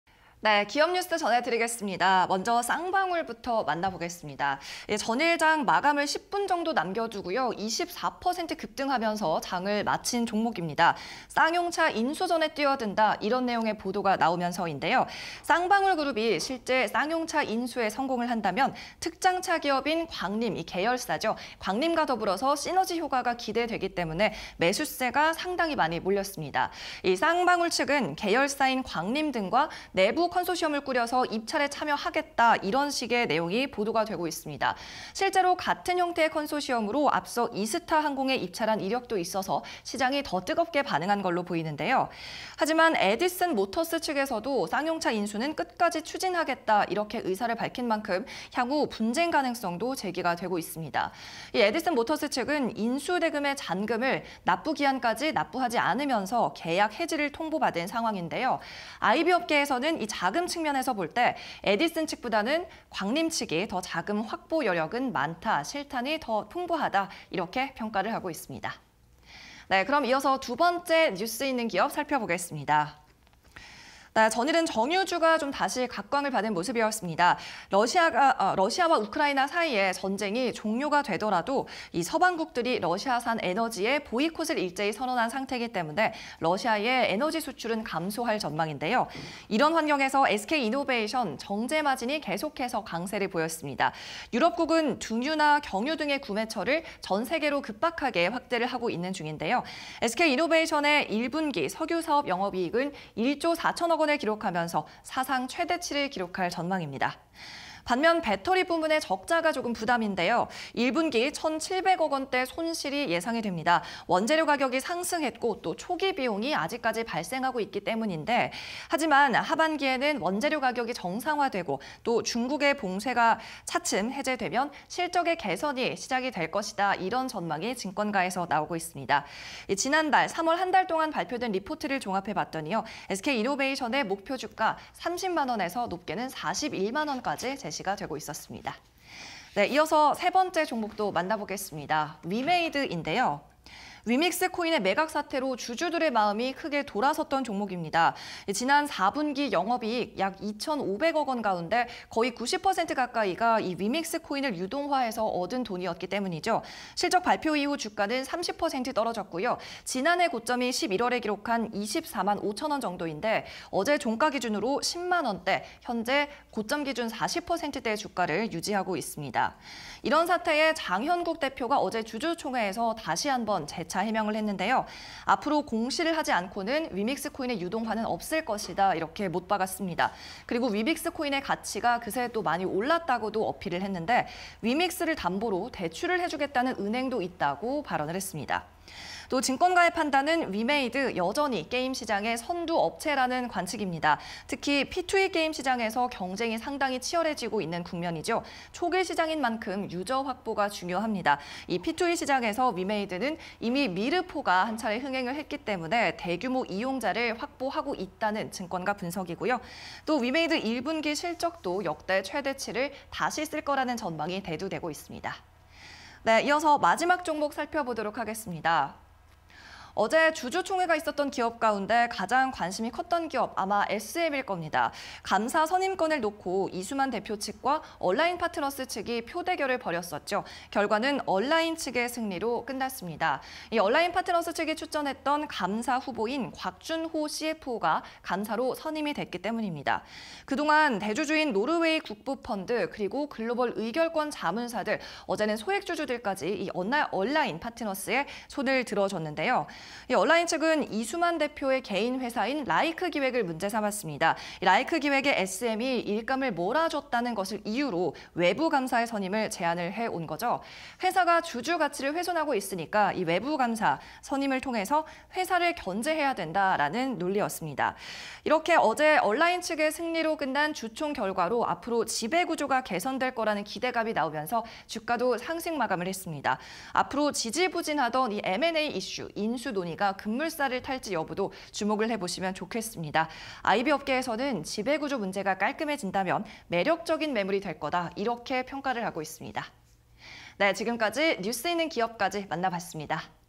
쌍방울, SK이노베이션, 위메이드, 에스엠_공시&뉴스 (20220401)
- 에디슨EV 고배 마신 ‘쌍용차 인수’ 시도
- 광림 등 계열사 컨소시엄 꾸릴 전망
- 에디슨모터스 “자금조달 가능”
- 양사 분쟁 가능성… 쌍방울, 자금 측면 유리

- 러-우크라 리스크 예상보다 장기화
- 석유사업 1분기 사상 최대치 예상
- 배터리, 영업적자 지속 예상

- 위믹스코인 매각으로 주주 신임 잃어
- 대표 “공시 없는 유동화 없다” 해명
- 증권가 “여전히 P2E 선두주자”
- '미르4' 흥행으로 초기 유저 확보

- 감사 선임 표 대결서 얼라인에 패배
- 얼라인 측 제안 ‘곽준호 CFO’ 감사로 선임
- '라이크기획' 용역 문제로 지적
- 지배구조 개선 기대감에 주가 상승